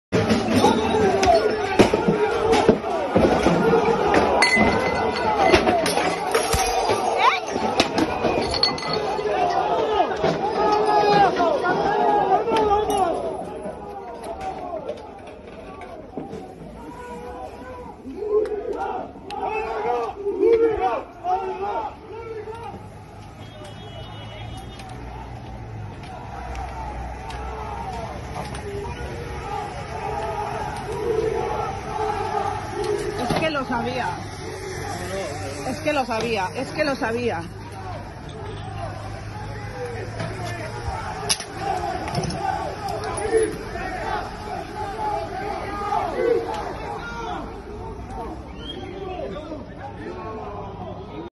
Nuevos enfrentamiento entre radicales del Racing y el Málaga: "Mi hija de 4 años estaba conmigo"
Un grupo de violentos, como ya ocurrió en el partido frente al Oviedo, han empañado la previa de un partido del Racing. Esta vez ha sido en las inmediaciones de Piquío, a donde acababa de llegar un grupo de seguidores del Málaga y donde se encontraron con otras personas con distintivos del Racing para provocar una pelea.